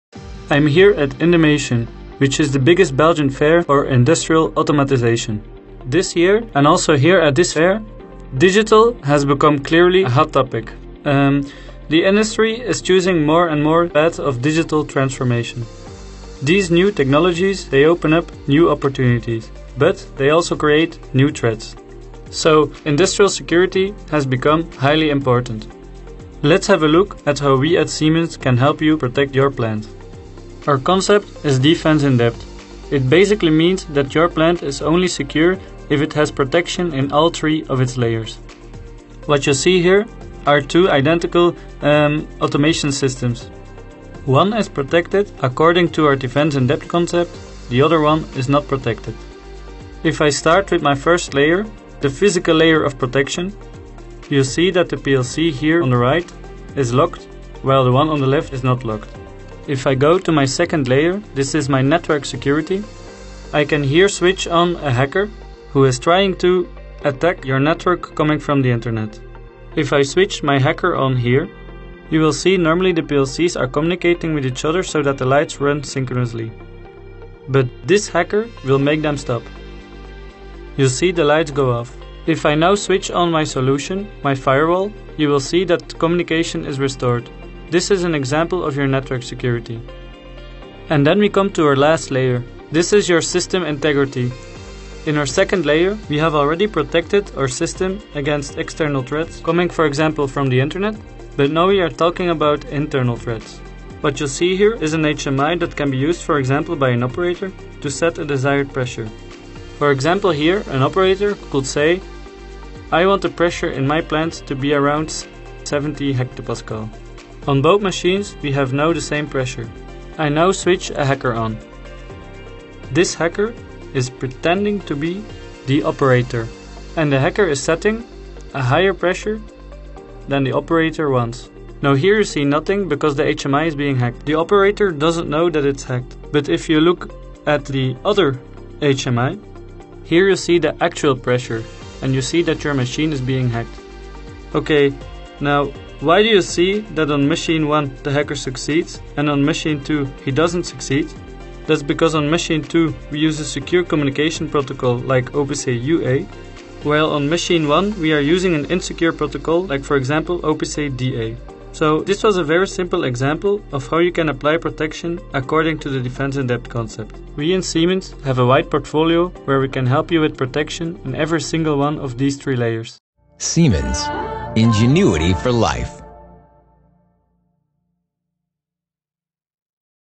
Siemens explains Defense in Depth - Indumation 2019
Industrial security is an essential element of Digital Enterprise, the Siemens way to Industry 4.0. With Defense in Depth, Siemens provides a multi-layer concept that gives your industrial plant both all-round and in-depth protection. The concept is based on plant security, network security and system integrity as recommended by ISA 99/IEC 62443. Dive in the concept with Augustijn Degrieck.